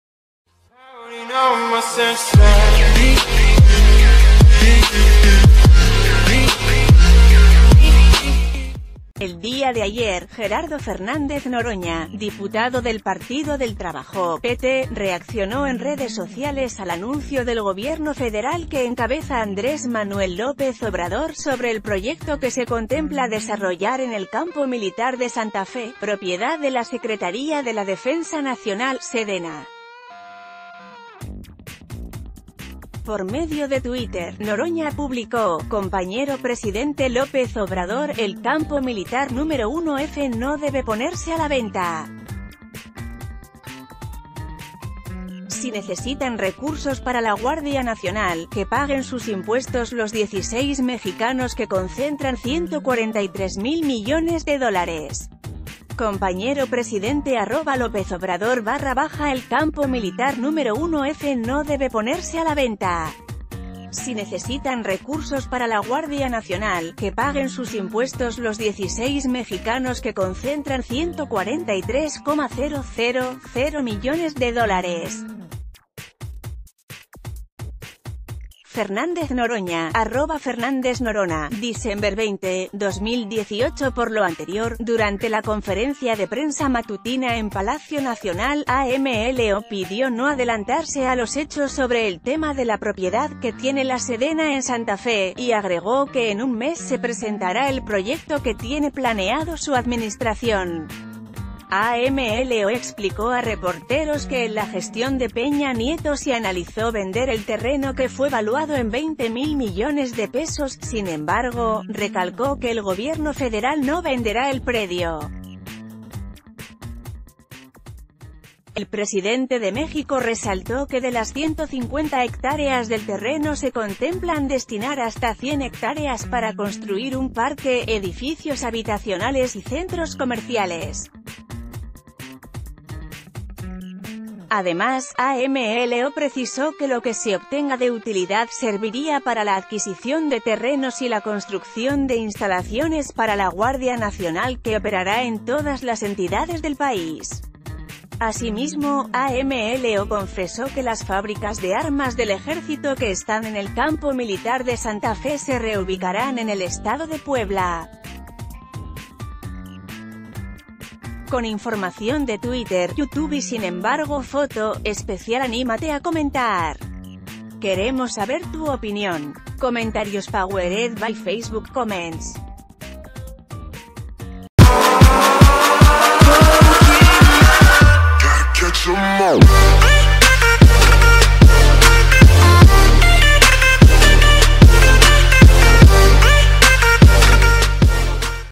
¡Mano dura! AMLO expone error de Noroña y pide no difundir noticias falsas
El día de ayer, Gerardo Fernández Noroña, diputado del Partido del Trabajo (PT), reaccionó en redes sociales al anuncio del Gobierno Federal que encabeza Andrés Manuel López Obrador sobre el proyecto que se contempla desarrollar en el Campo Militar de Santa Fe, propiedad de la Secretaría de la Defen...